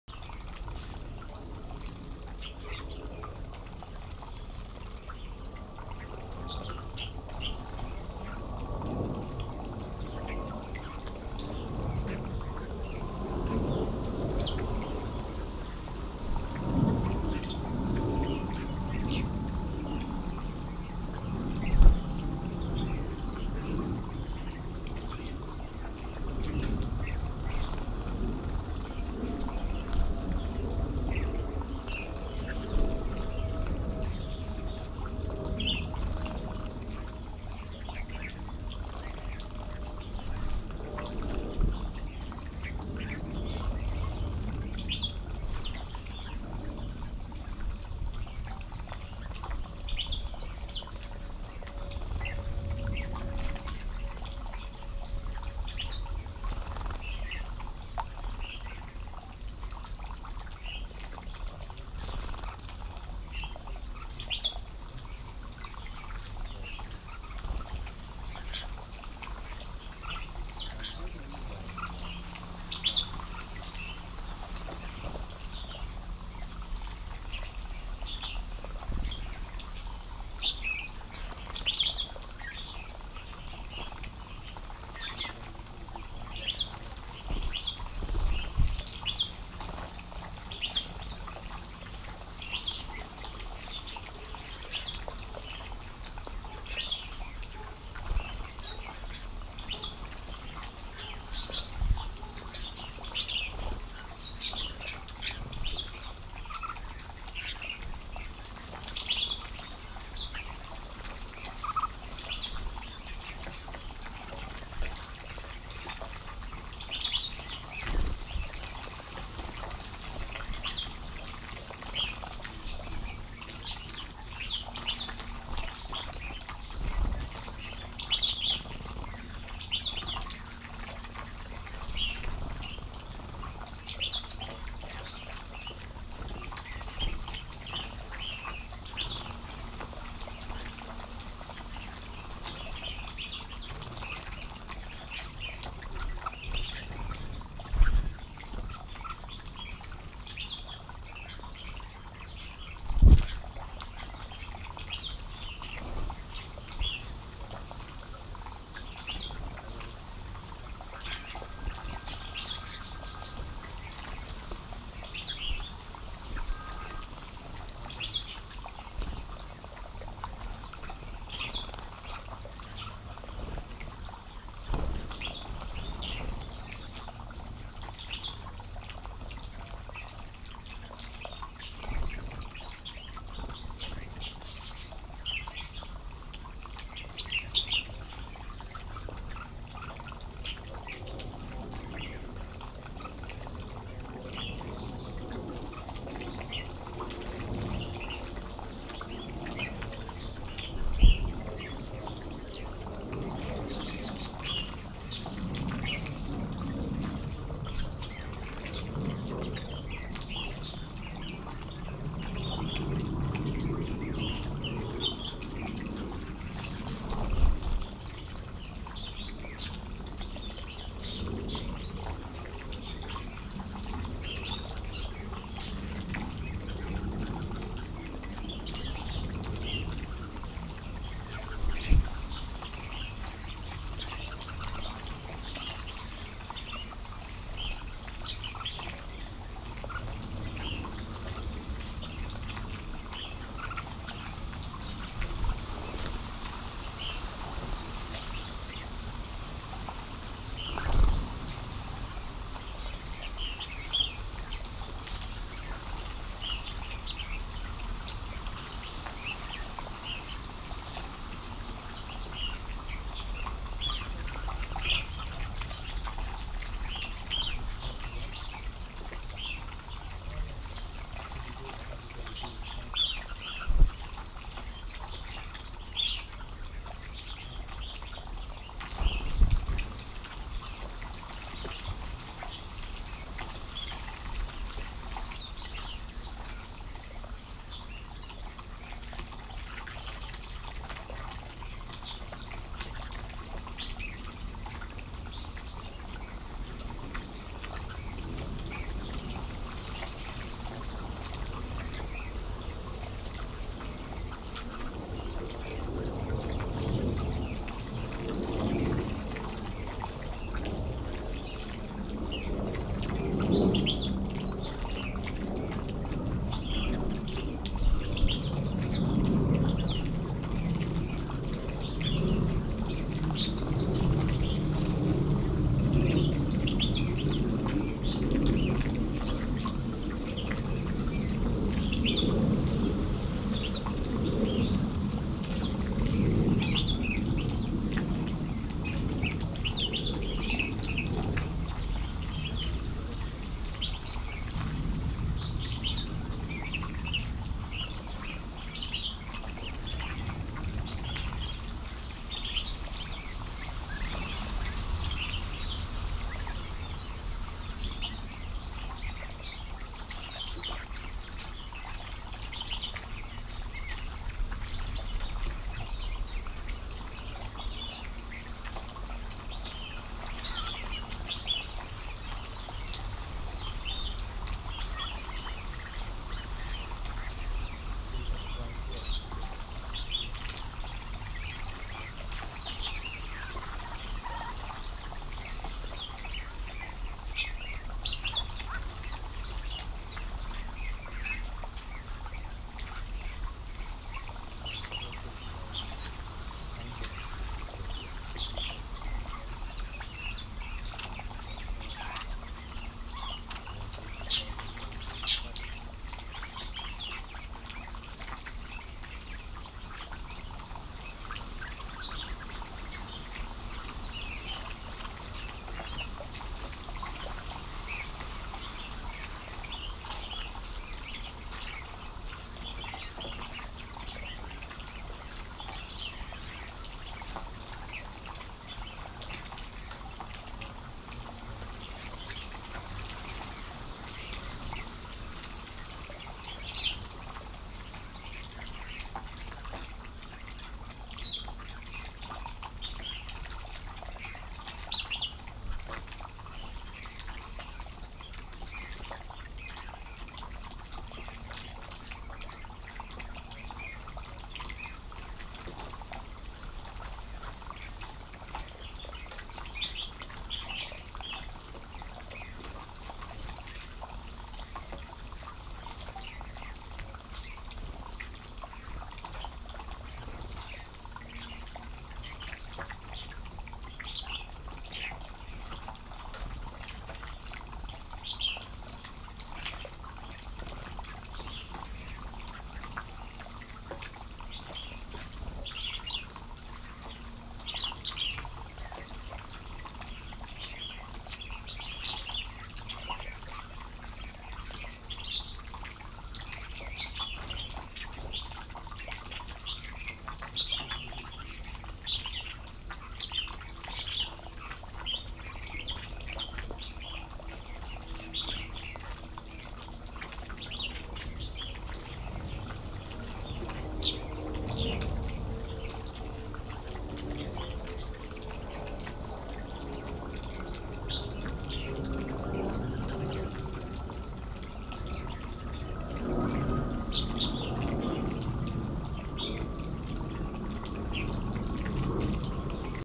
Bulbuls Pond Lamma HK GoPro4 2 7k aud 28Nov 2015
This is about an 8 min video shot by a GoPro4 off a bush pond on a hillside on Lamma Island, Hong Kong. Winter visitors from north Asia are arriving, including thrushes and flycatchers. Mostly though you are seeing bulbuls - Red-whiskered and Chinese.